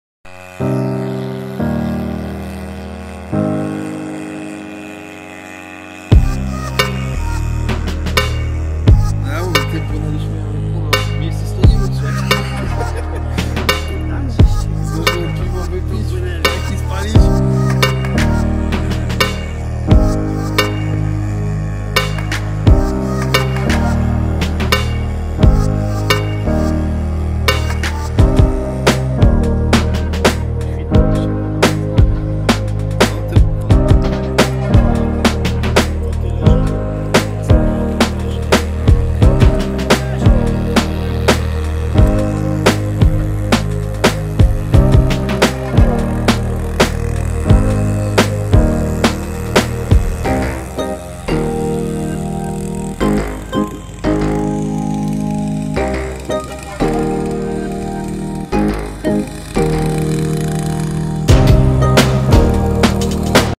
Delphin 140 Odra 26.06.2018 Pierwsze wodowanie test silnika spalinowego
Pierwsze wodowanie   test silnika spalinowego